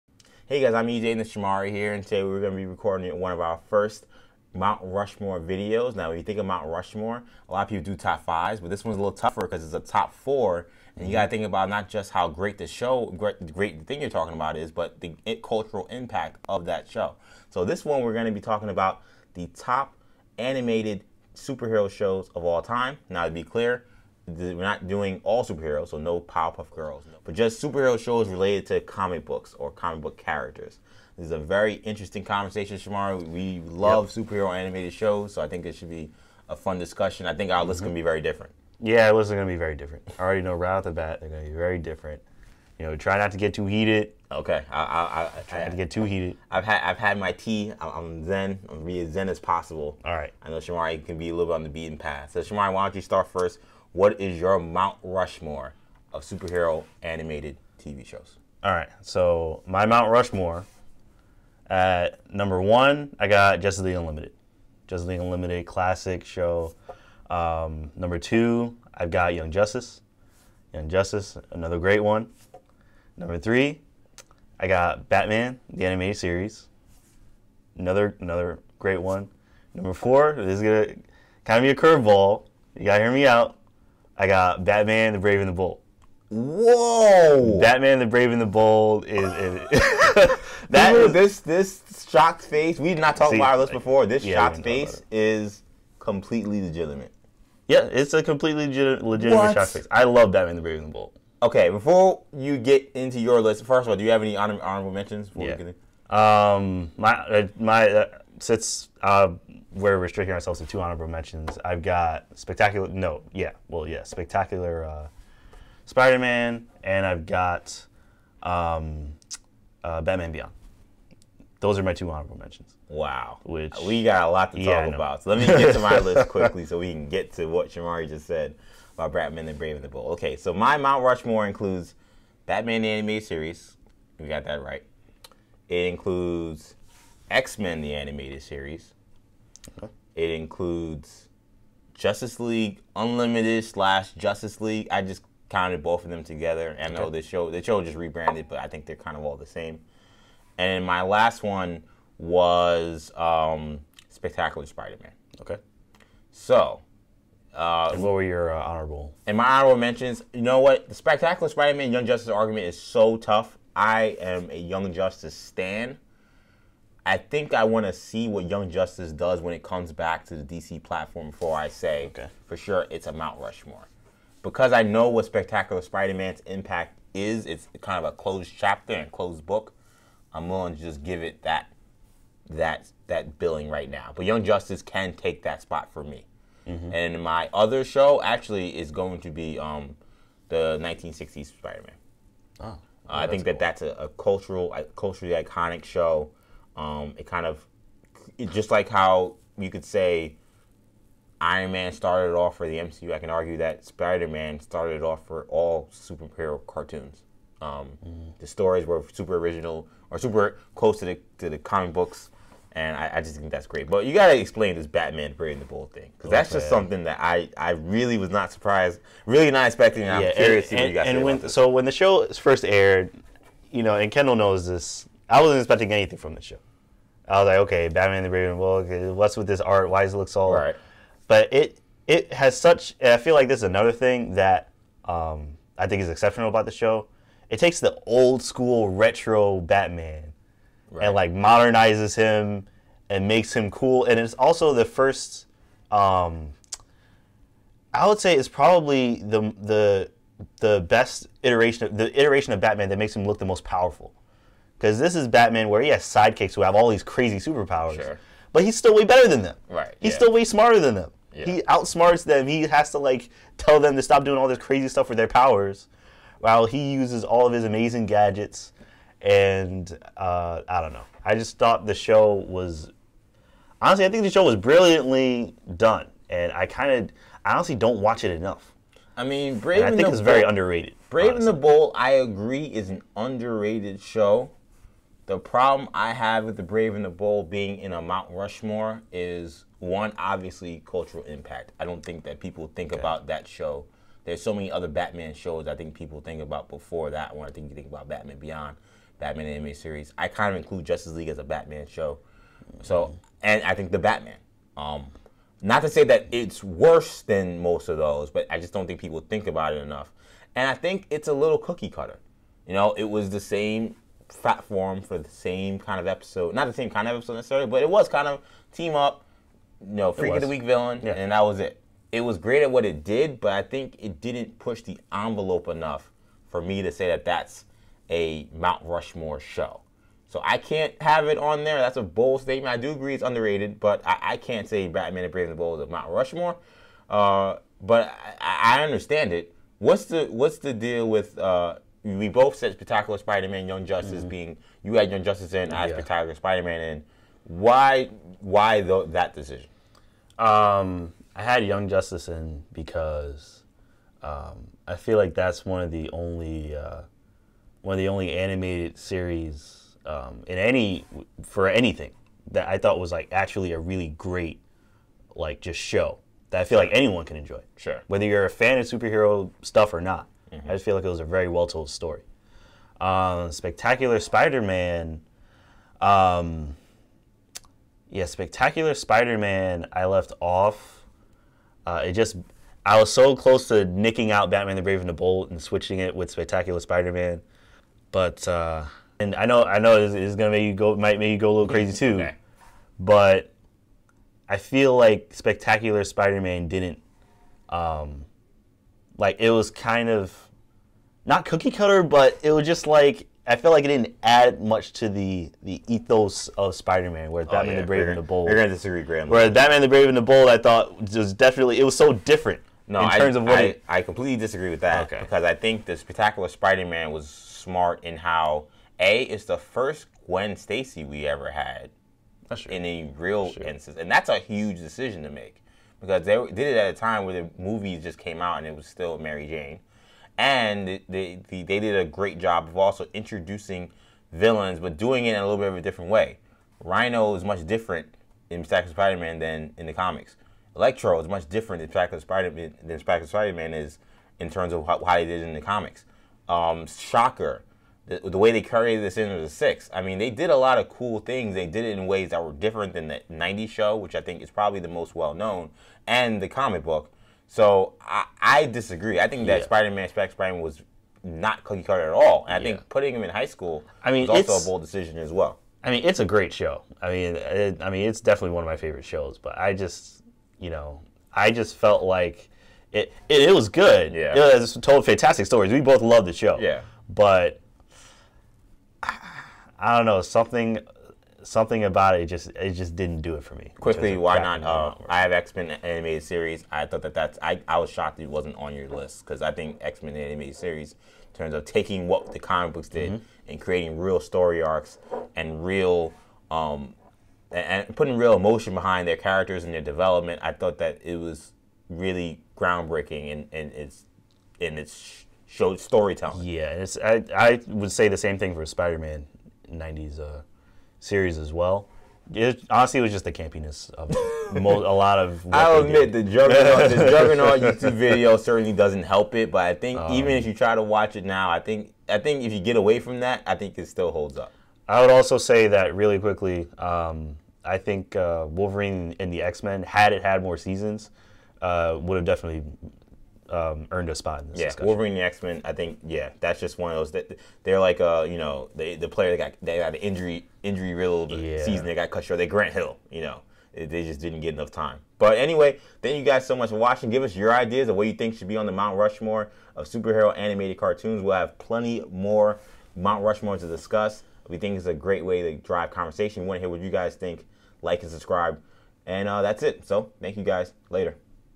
Mount Rushmore: Top Animated Superhero Shows
In the first video of our Mount Rushmore Series, EJ and Shamari debate over the greatest superhero cartoons of all time.